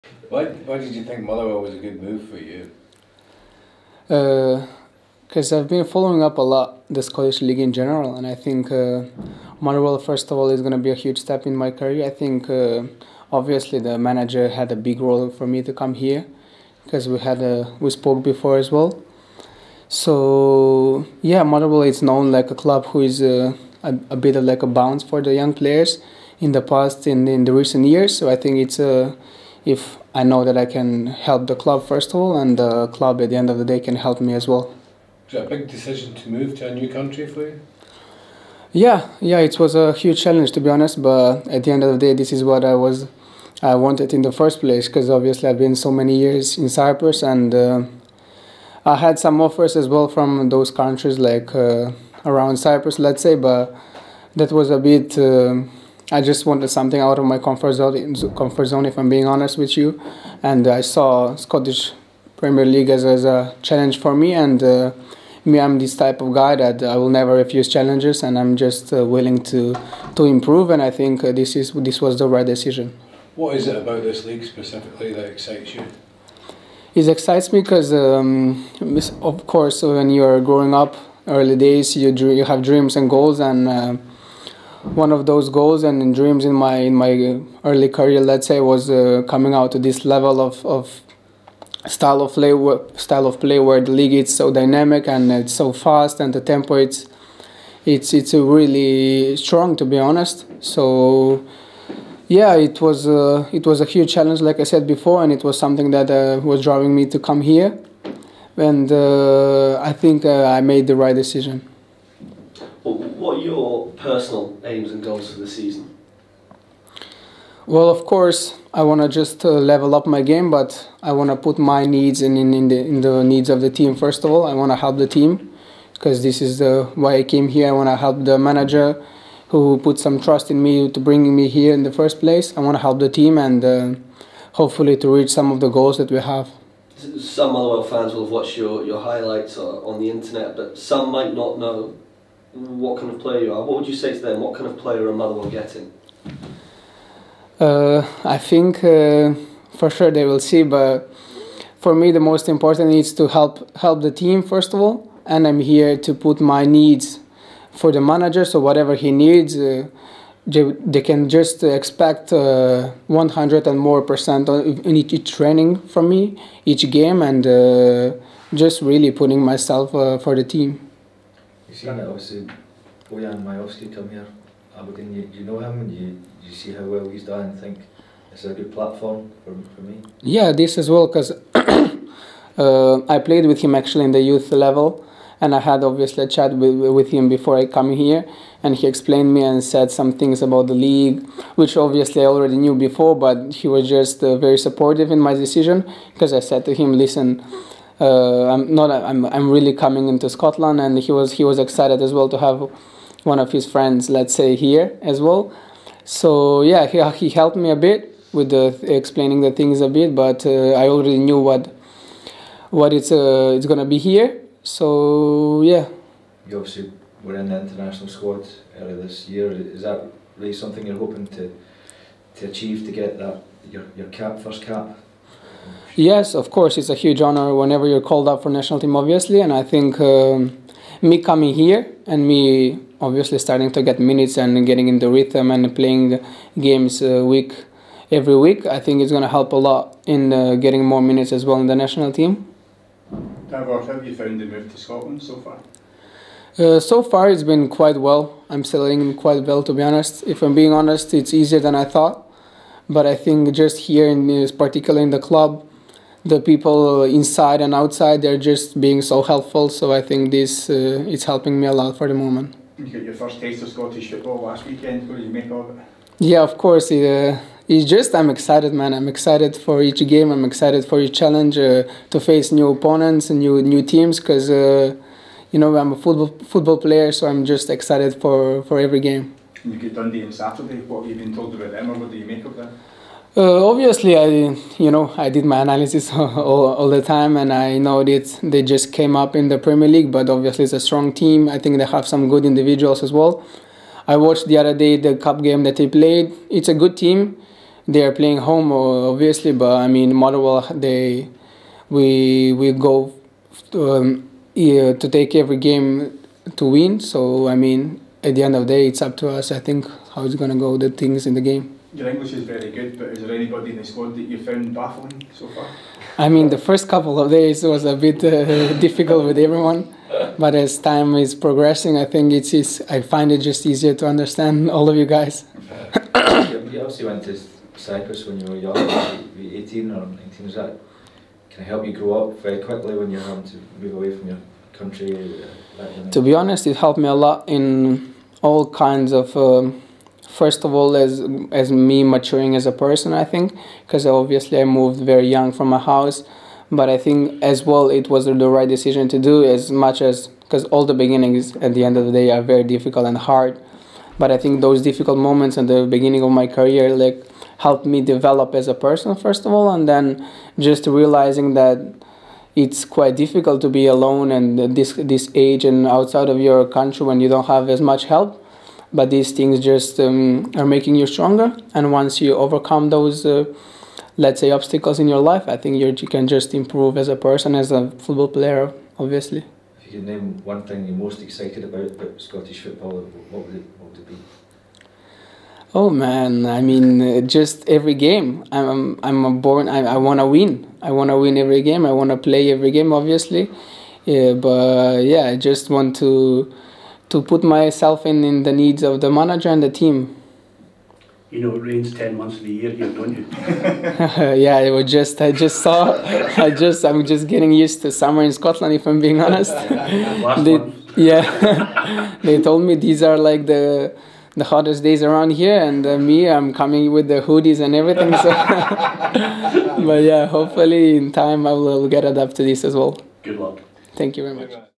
Davor Zdravkovski previews Dundee trip
Davor Zdravkovski made his Motherwell debut on Saturday and is now raring to go ahead of the new cinch Premiership campaign.

The Macedonian discusses how quickly he has settled in thus far in Scotland and how he feels it has been easier than he initially anticipated.

Zdravkovski wants to help the team as much as possible this season but also wants to repay the faith shown in him by Stuart Kettlewell.